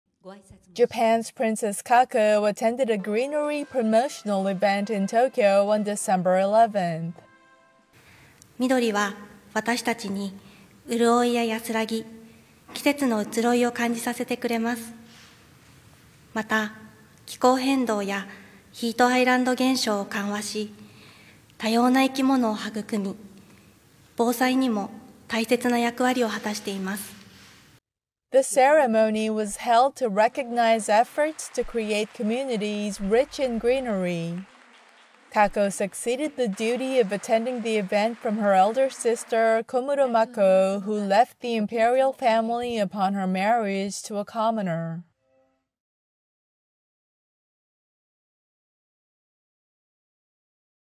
Princess Kako attends greenery event in Tokyo
Japan's Princess Kako attends a greenery promotional event in Tokyo and shares her views on greens and plants.